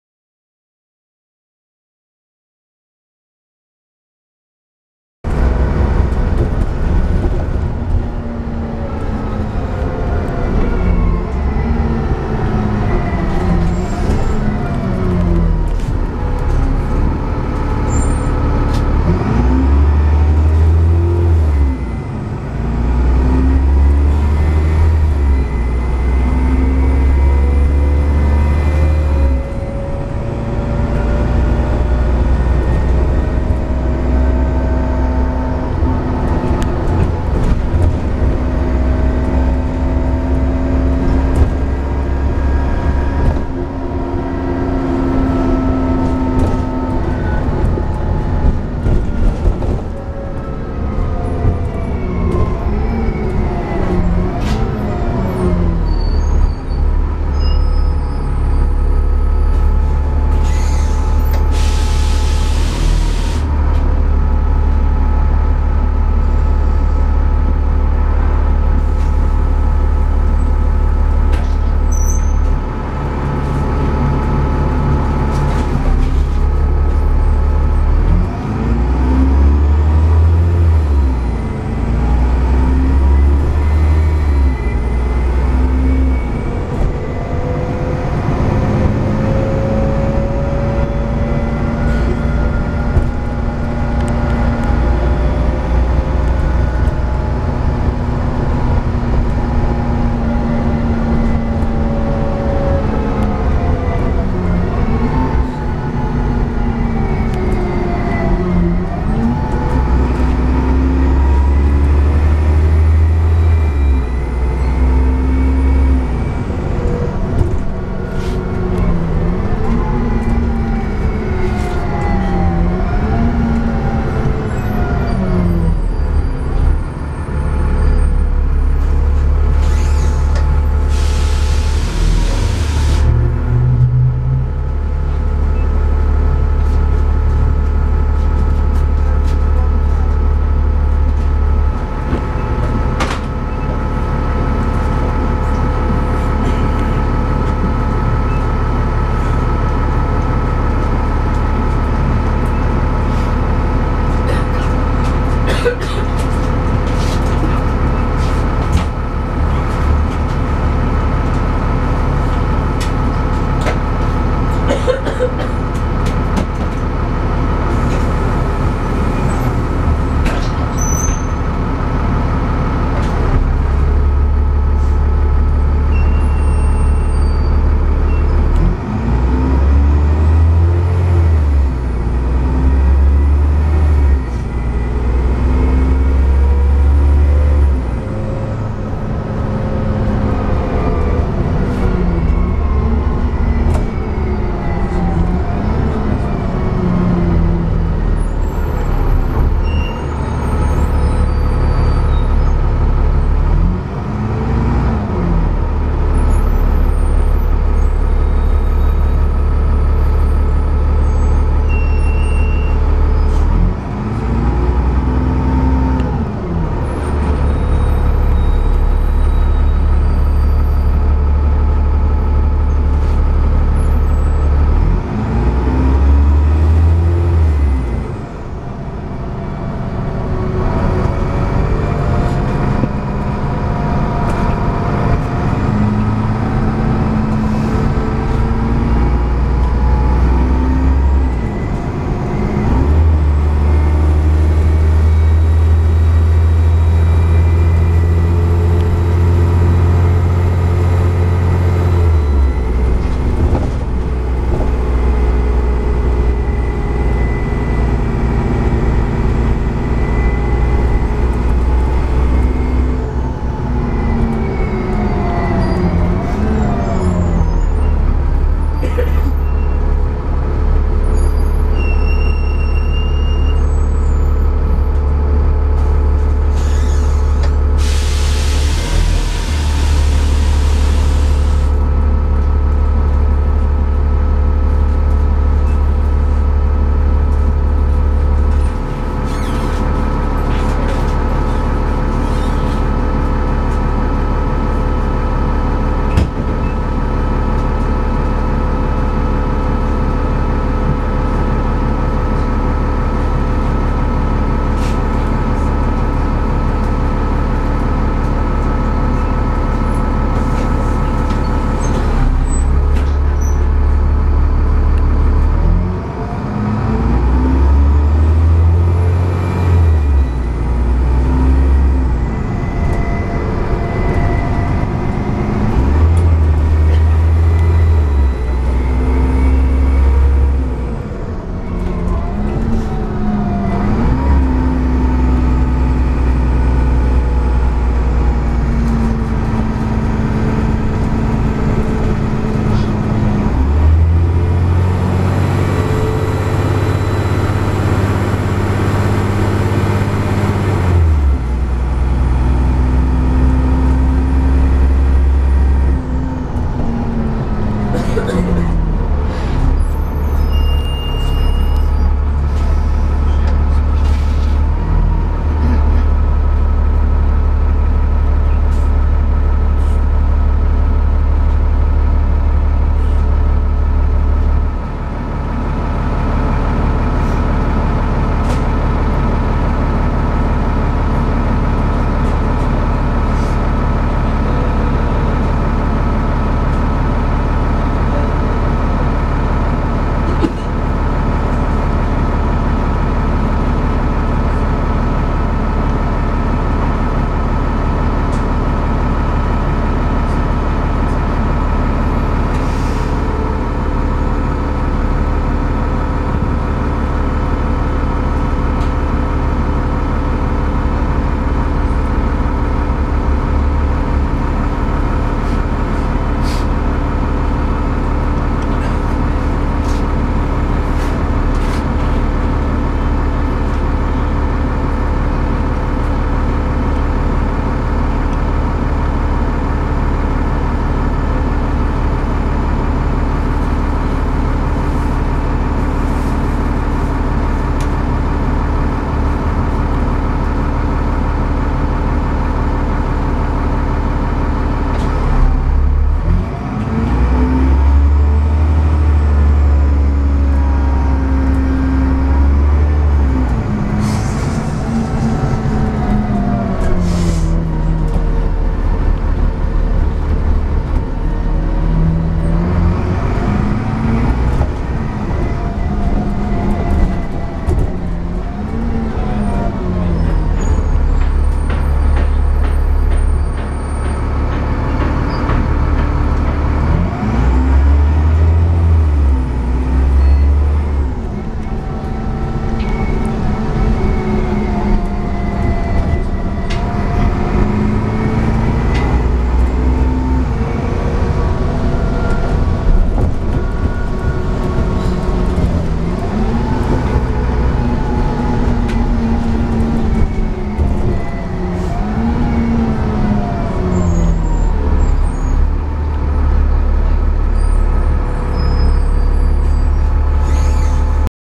Stagecoach in Eastbourne Trident 18174 GX54 DVT
TransBus Trident/ALX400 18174 GX54 DVT working the 1A 12/2/16 created this video with the YouTube Video Editor